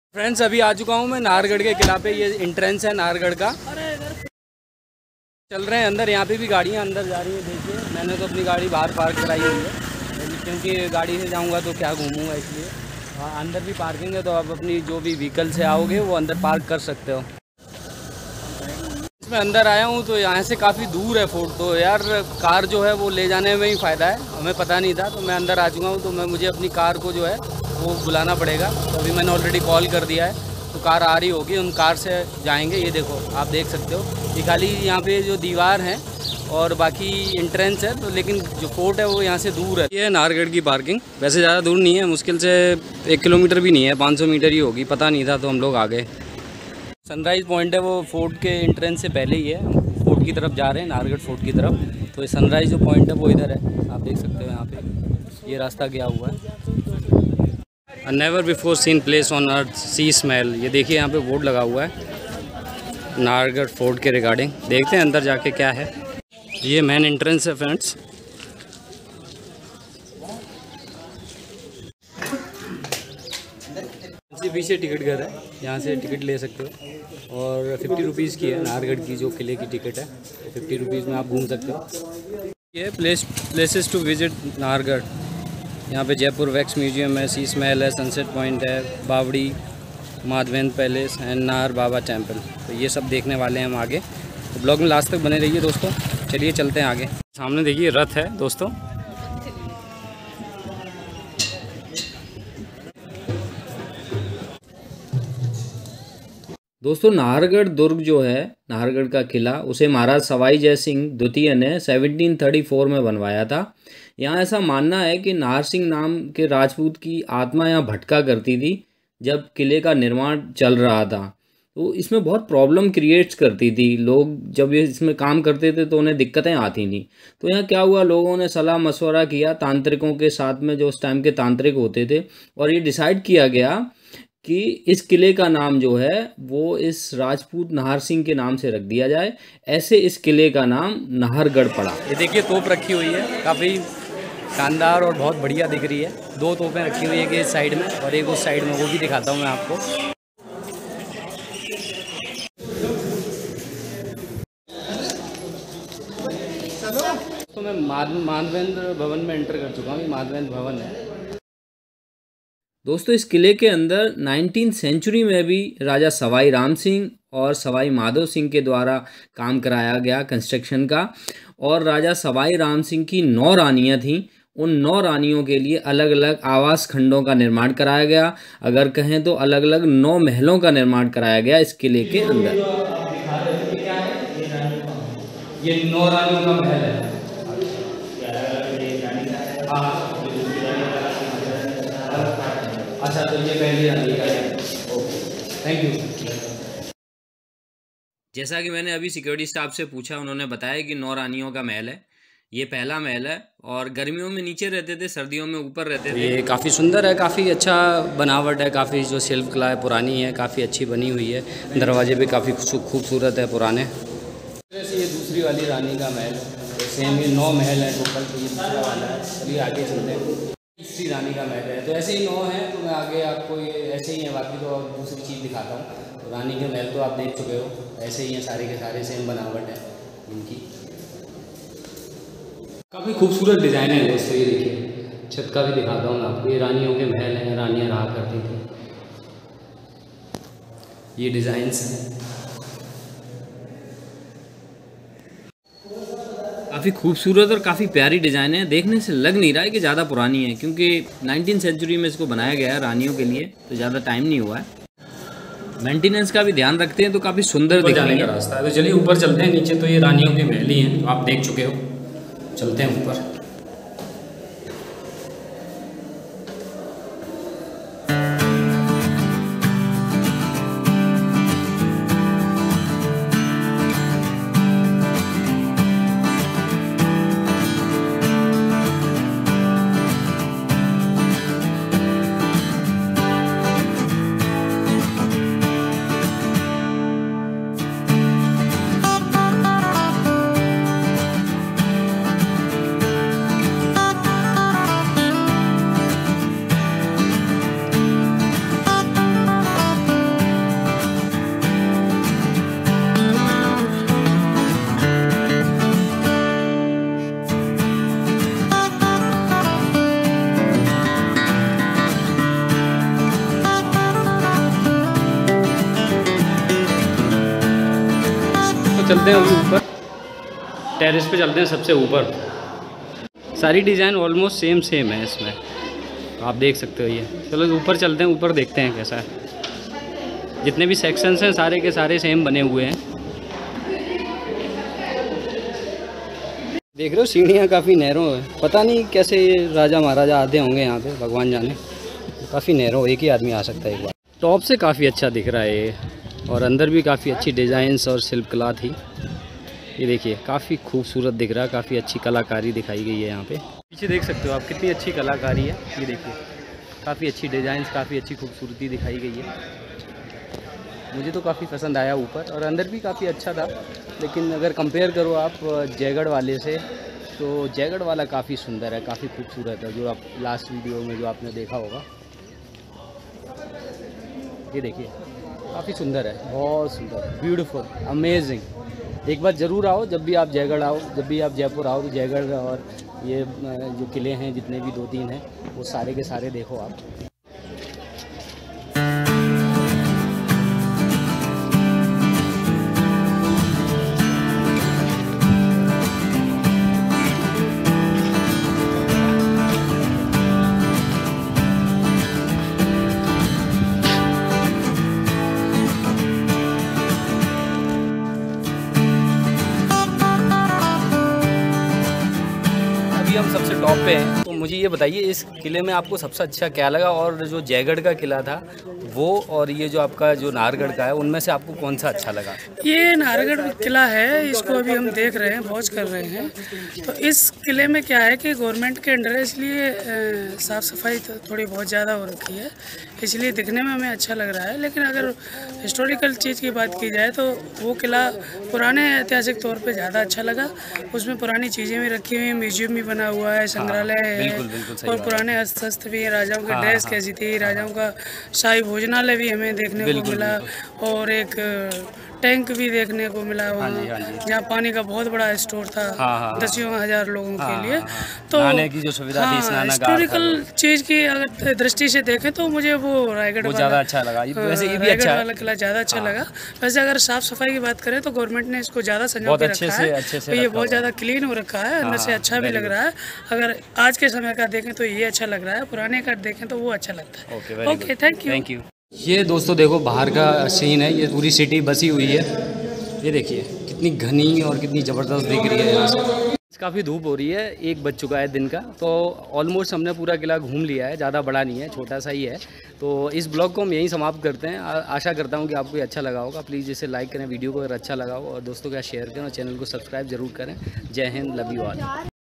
Nahargarh Palace Jaipur | Nahargarh fort Jaipur Rajasthan | नाहरगढ़ का किला जयपुर| Hindi Vlog
Nahargarh Fort stands on the edge of the Aravalli Hills, overlooking the city of Jaipur in the Indian state of Rajasthan. Along with Amer Fort and Jaigarh Fort, Nahargarh once formed a strong defence ring for the city. The fort was originally named Sudarshangarh, but it became known as Nahargarh, which means 'abode of tigers'.
The popular belief is that Nahar here stands for Nahar Singh Bhomia, whose spirit haunted the place and obstructed construction of the fort. Nahar's spirit was pacified by building a temple in his memory within the fort, which thus became known by his name.